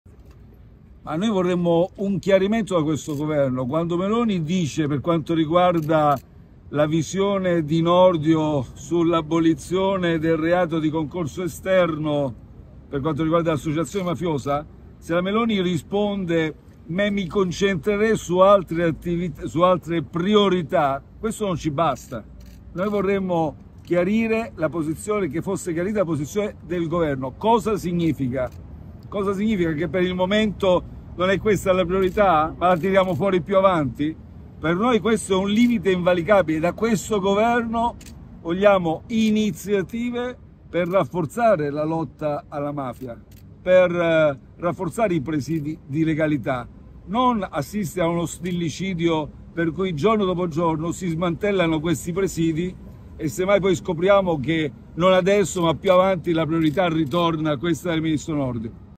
Conte su concorso esterno: «Governo chiarisca, stillicidio per smantellare presidi di legalità»
Le dichiarazioni del presidente del M5S, Giuseppe Conte, a margine del convegno 'La rivoluzione ecodigital' nella sala Matteotti di Palazzo Theodoli Bianchelli.